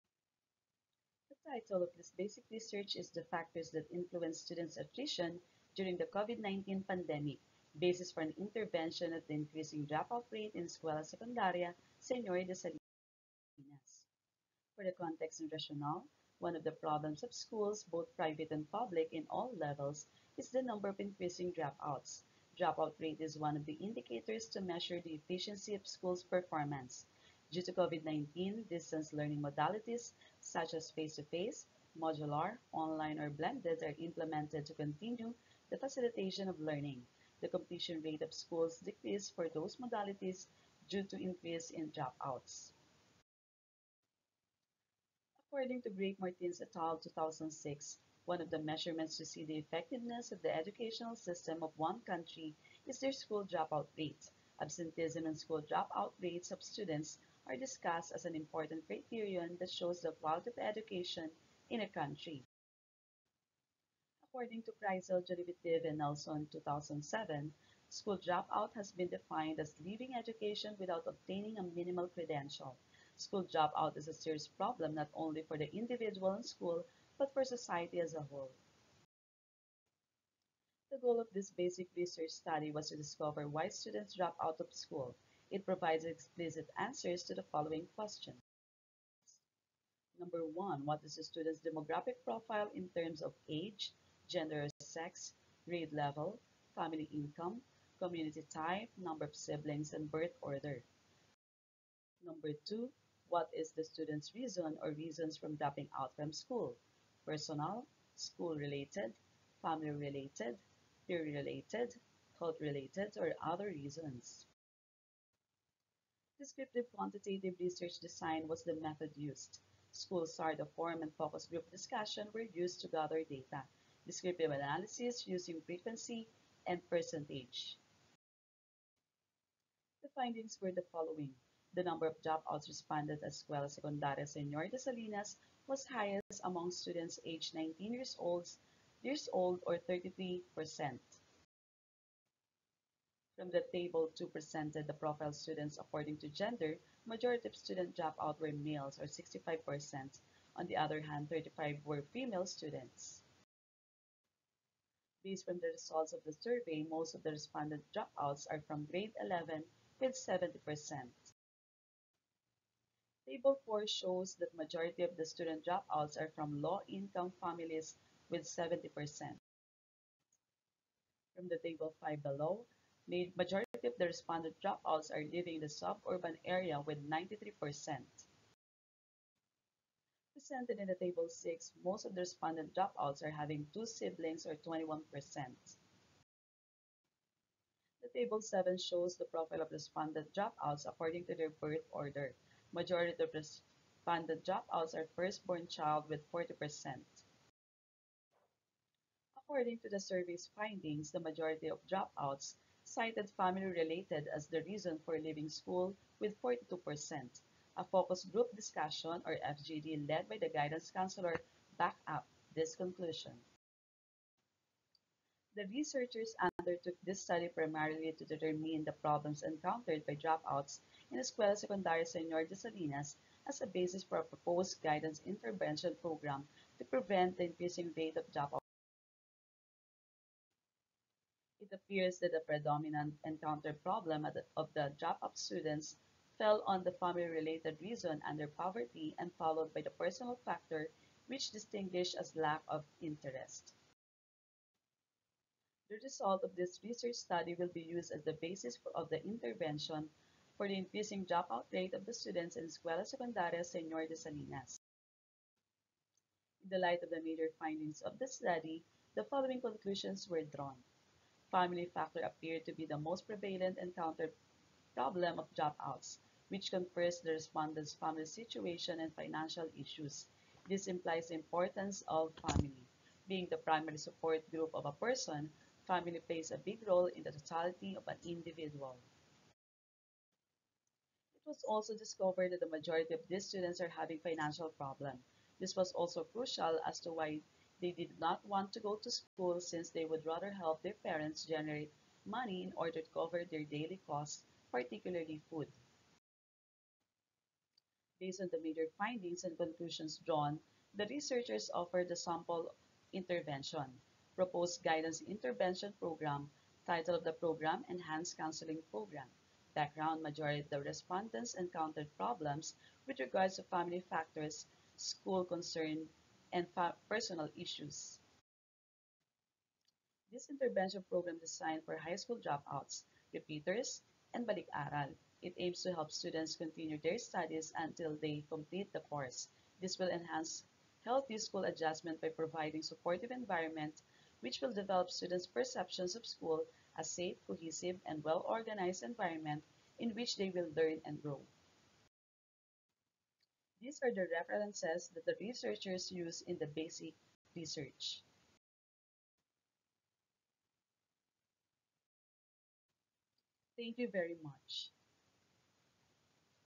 FACTORS THAT INFLUENCE STUDENTS' ATTRITION DURING THE COVID-19 PANDEMIC
FACTORS THAT INFLUENCE STUDENTS' ATTRITION DURING THE COVID-19 PANDEMIC: BASIS FOR AN INTERVENTION OF THE INCREASING RATE  OF DROPOUT STUDENTS IN ESCUELA SECONDARIA  SEÑOR DE SALINAS
ROS-BR-GO-55-22
Marilyn B. Encarnacion, Ana Liza A. Dalangin, Evangeline A. Mercado

Purpose: The increasing number of dropouts in schools, both private and public, at all levels has become a pressing issue, particularly exacerbated by the COVID-19 pandemic. This Basic Research aims to identify the factors contributing to student attrition during the pandemic as an intervention to address the rising dropout rate.
Design/methodology/approach: Purposive sampling and descriptive research methodology were employed in this study. The respondents consisted of students who dropped out of school within the past two years. Data collection was carried out through a survey form to identify the factors influencing student attrition during the pandemic.
Findings: The study revealed that the primary factor contributing to student dropout was family-related issues, with 18 out of 43 respondents (42%) citing it as the main reason. Personal factors accounted for 15 respondents (35%), followed by school-related factors (17%), peer-related factors, and health-related factors, both at 5% of the total respondents. Financial difficulties emerged as a crucial factor, as many students had to forgo school to support their families in meeting daily expenses, particularly food. Lack of interest was also a significant personal reason for dropout, emphasizing the need for parental and teacher supervision and support.
Research limitations/implications: It should be noted that not all dropout students from the previous year were able to participate and respond to the survey form, potentially limiting the representation of all dropouts in the ESSDS. Based on the study's findings, an intervention program titled "Enhanced Counseling Program" was conceptualized to address and prevent dropout cases. This program aims to target the three prevalent factors: personal, family, and school-related. By offering guidance and support in these areas, the program aligns with the Department of Education's goal of providing better and quality education to Filipino youth.
Originality/value: Mentoring by experienced teachers, seeking advice from research specialists, and collaborating with seasoned practitioners can enhance the research process in addressing dropout issues. The study's information gathering is based on the Students At Risk of Dropping Out (SARDO)/No Longer Participating in Activities (NLPA) form. This mentorship approach provides personalized guidance, insights from experts, and assistance from experienced practitioners in tackling the challenges related to dropout research.

Want to replicate/adapt/cite this research? Let us know by filling out this